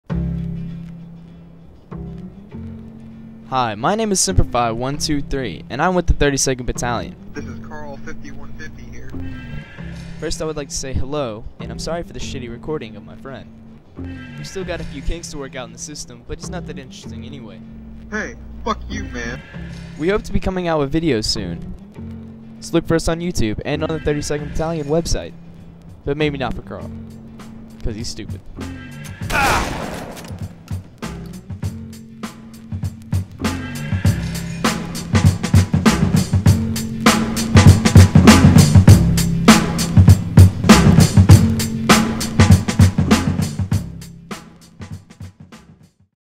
32nd Battalion Miniseries Announcement
This is a 32nd Battalion internet series/machinima announcement video.
Starring Members : Semperfi123 and Carl5150
Filmed And Directed By : Semperfi123